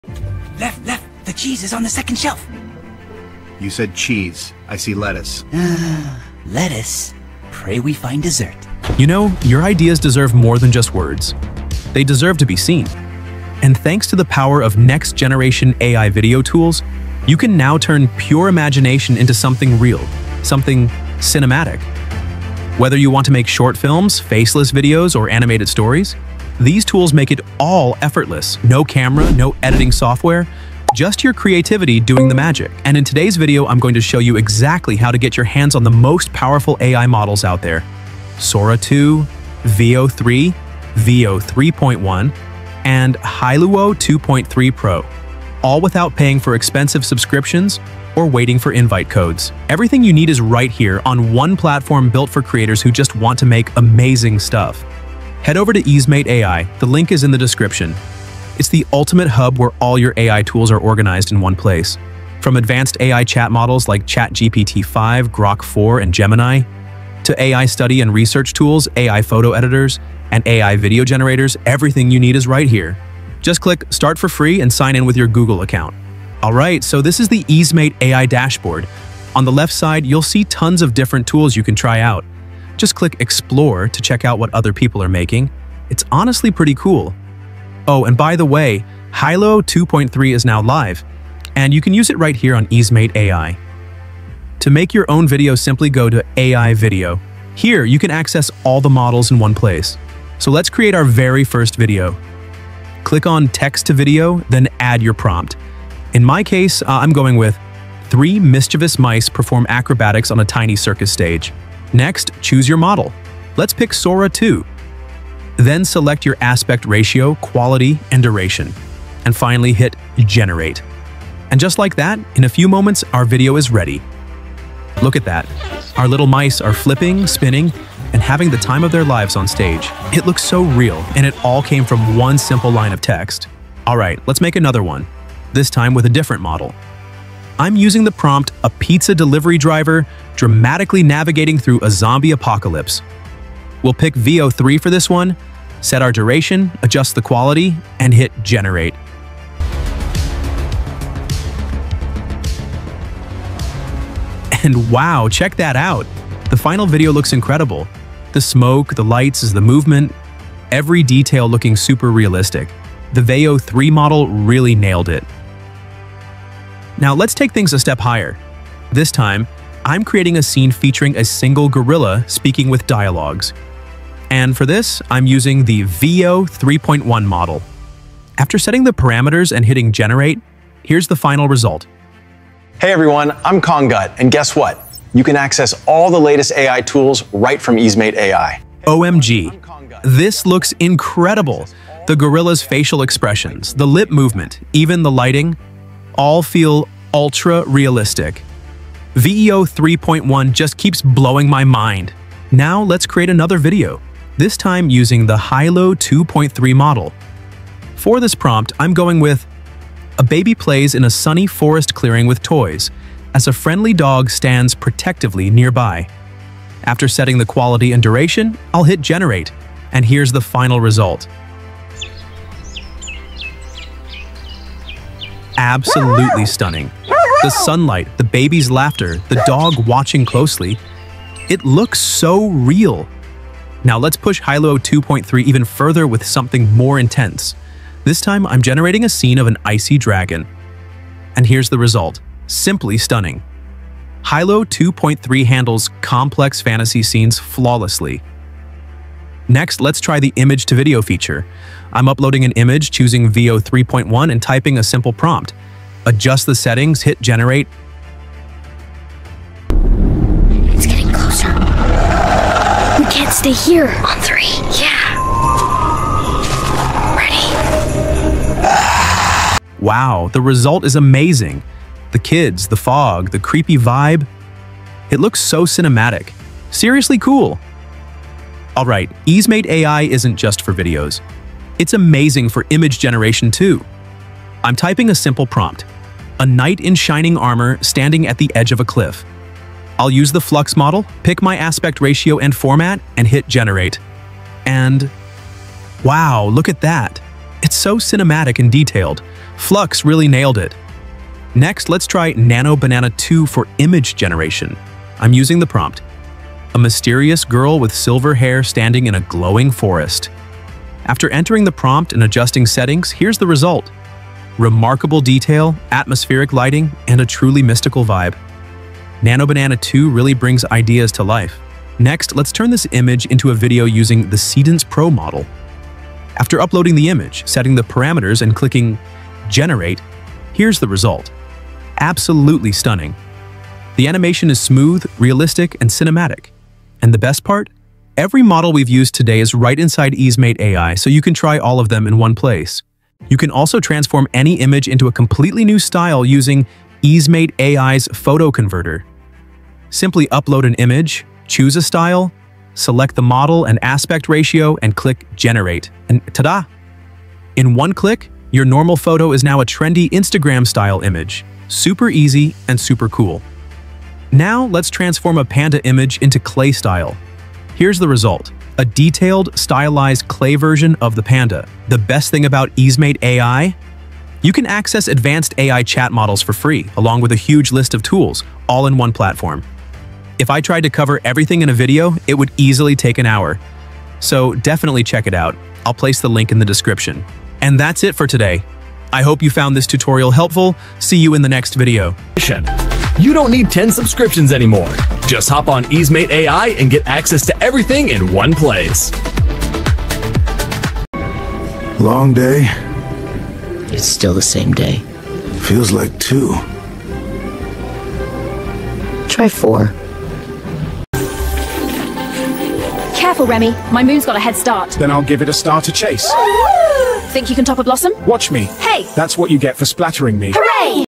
Forget Higgsfield — This New AI Website Lets You Use Sora 2 & Veo 3.1 for Free 😱
Forget Higgsfield! This FREE AI website gives you access to Sora 2, Veo 3.1, Hailuo 2.3, and more — create cinematic AI videos in minutes! 😱

Discover EaseMate AI – the ultimate all-in-one AI platform where creativity meets technology. Get free access to top-tier AI models like Sora 2, Veo 3.1, Hailuo 2.3, Nano Banana 2, Flux, Seedence Pro, and more — all in one place.

Whether you’re making cinematic AI videos, text-to-video animations, image-to-video transformations, voiceovers, avatars, or visual effects, EaseMate lets you bring your ideas to life instantly.

No subscriptions. No invite codes. No complicated setup. Just type your prompt, choose your model, and generate stunning, high-quality videos and images within minutes.
Explore Flux and Seedence Pro for high-quality visuals

🔹Key Features:

• Free access to top AI models: Sora 2, Veo 3.1, Hailuo 2.3, Nano Banana 2

• Create cinematic AI videos from text

• Turn images into videos instantly

• AI image generation & style conversion (Instagram, clay, etc.)

• Access advanced AI chat models for free

• All-in-one platform, no subscriptions or invite codes

• Fast, high-quality results